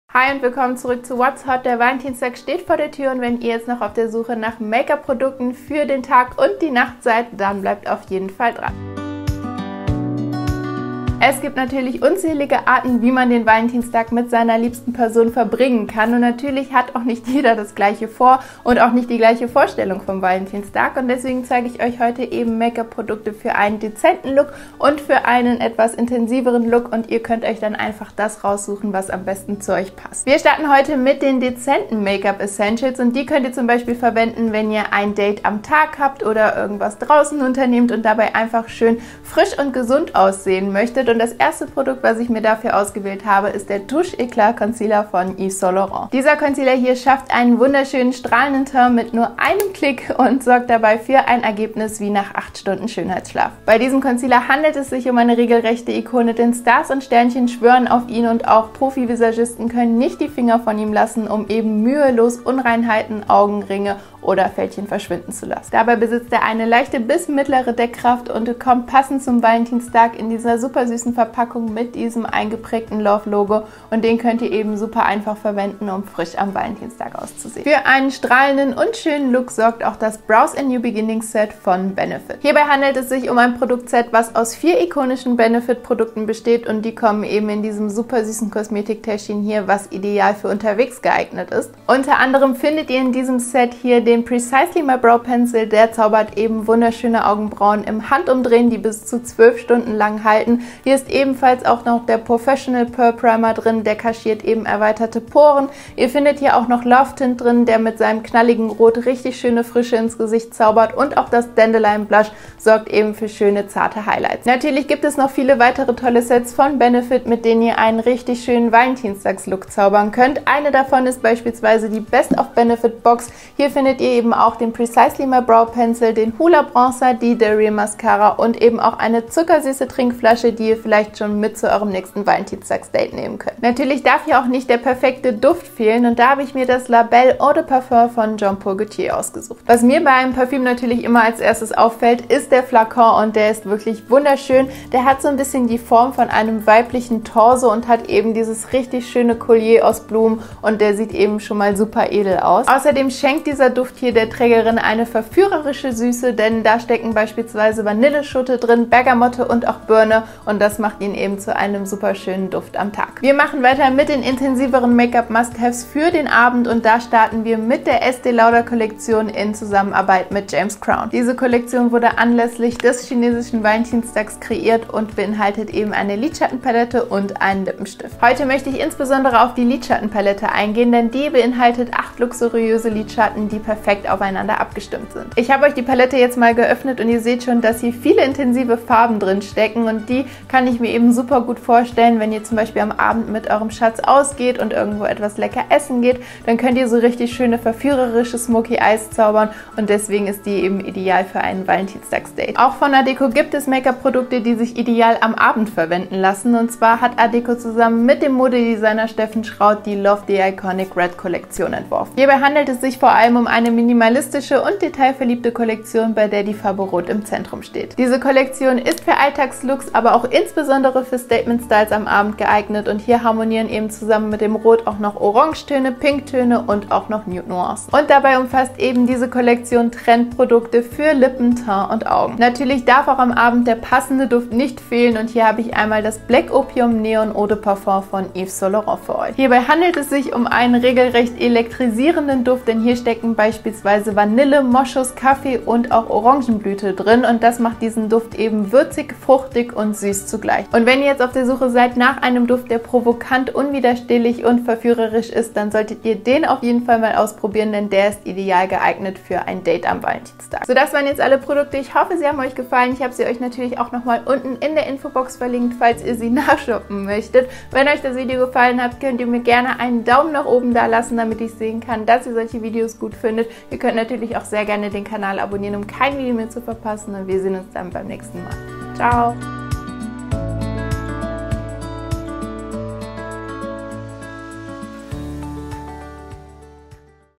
Valentines Beauty Must-haves I dezent & intensiv - YSL, Benefit & co I
Beauties aufgepasst!

Ihr seid noch auf der Suche nach den perfekten Make-up Produkten für den Valentinstag?
In unserem heutigen YouTube Video zeigt euch  Maike unterschiedliche Beauty Essentials, mit denen ihr sowohl einen natürlichen Look für den Tag als auch einen intensiven Make-up Look für die Nacht kreieren könnt.

•      Benefit Brows & New Beginnings! Make-up Set
 (Precisely my Brow Pencil,  POREfessional Pearl Primer, Dandelion Blush, Lovetint)

Ihr könnt nicht genug von Beauty bekommen?